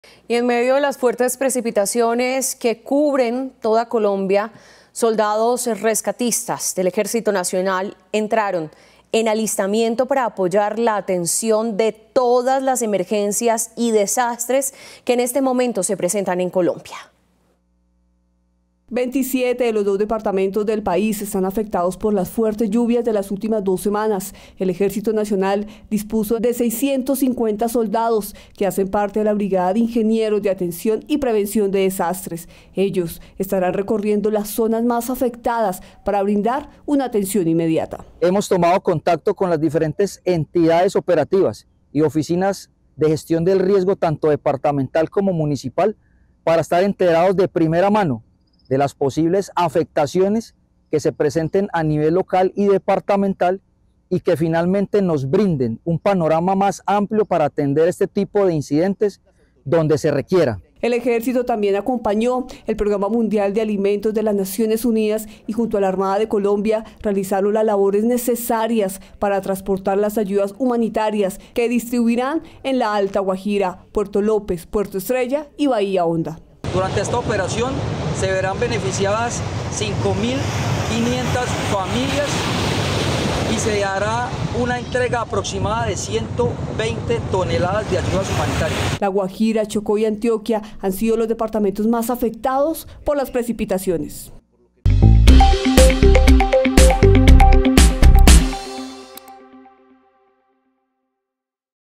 Fuertes lluvias afectan a 27 departamentos en todo el país
Afectaciones en 27 departamentos del país están afectados por las fuertes lluvias de las últimas semanas.